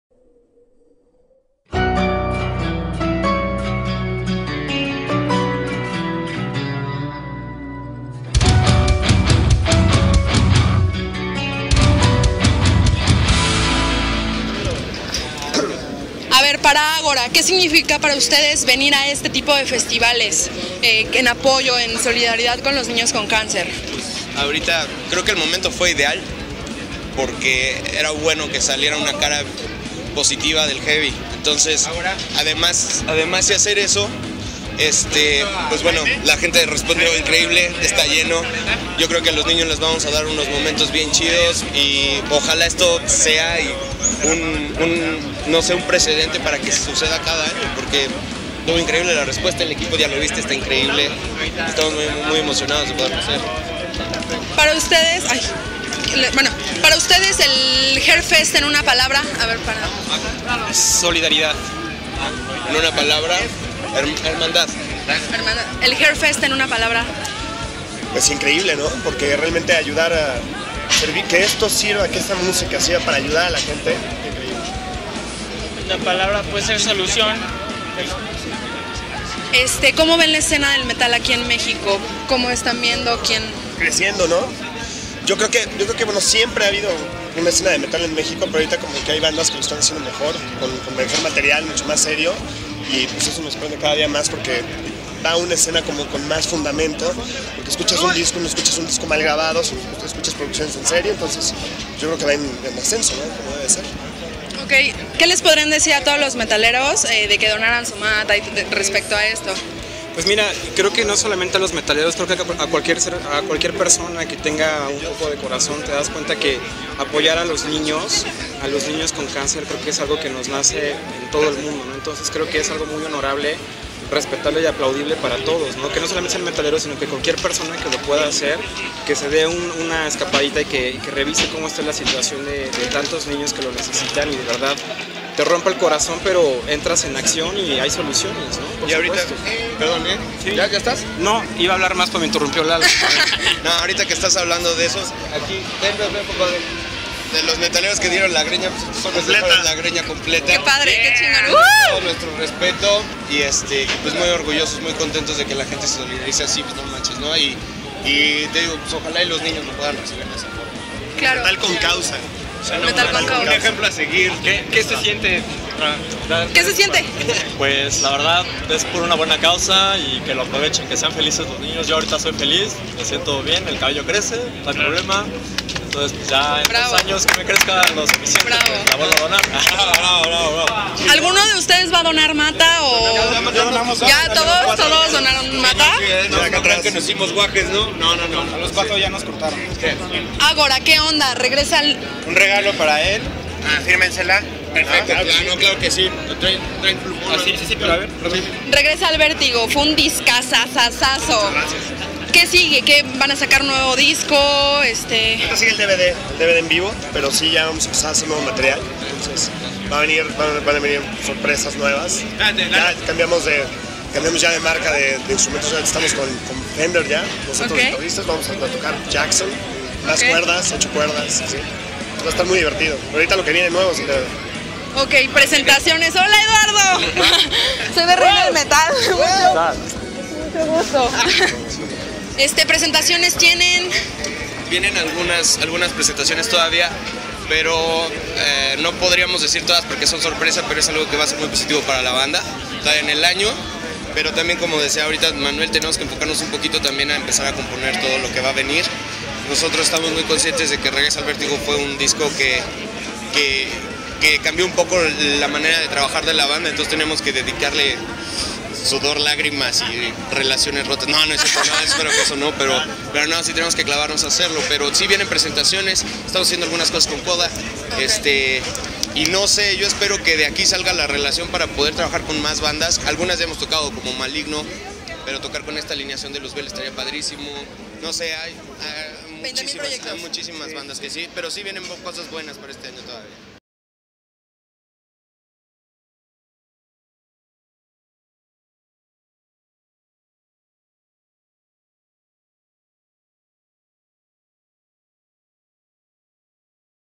Entrevista: Agora | HairFest
THE HAIR FEST el 12 de abril de 2014 en Polyforum Siqueiros, Ciudad de México.

Gracias a la organización, bandas, patrocinadores y a los asistentes que donaron cabello, convirtiéndose en los héroes anónimos del festival.

¡Nos vemos el año entrante!